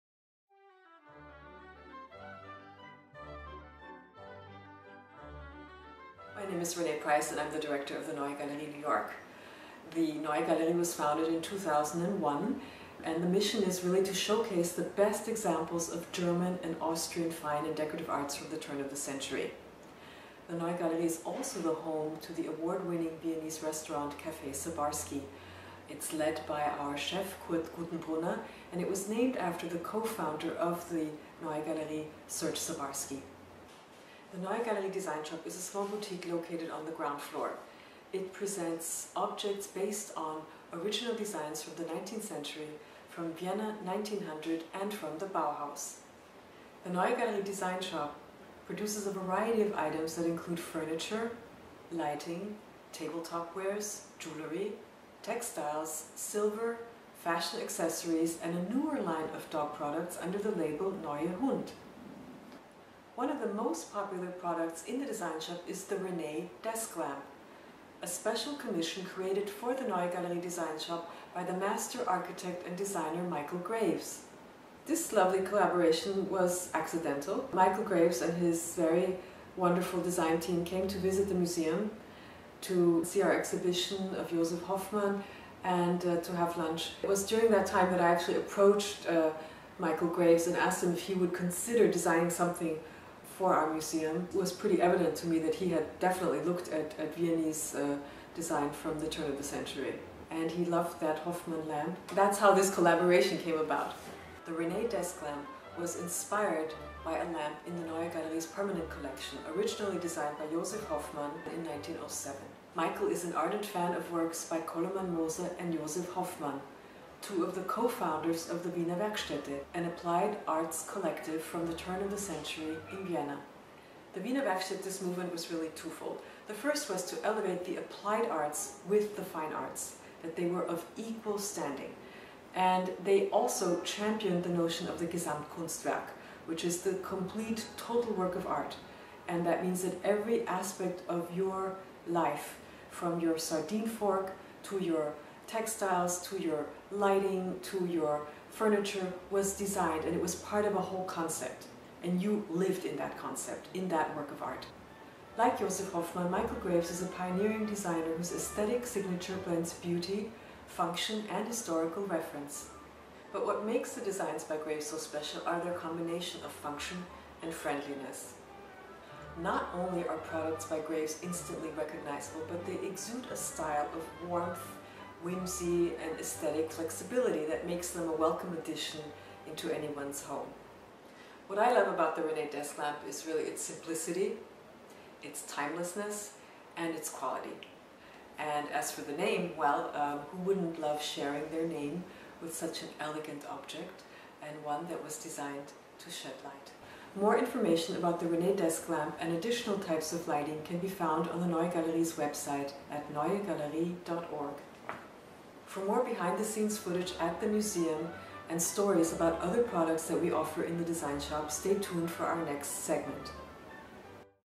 Neue Galerie Design Shop Video Exclusive: The Renée Desk Lamp
Neue Galerie New York Director Renée Price reveals the origins behind the "Renée Desk Lamp," a limited edition table lamp produced exclusively for the museum's design shop by the architect and designer Michael Graves.

Inspired by a lamp in the Neue Galerie's permanent collection designed by Wiener Werkstätte co-founder Josef Hoffmann in 1907, this interview also offers historical context about decorative objects, interiors and architecture created by members of this applied arts collective at the turn of the century in Vienna.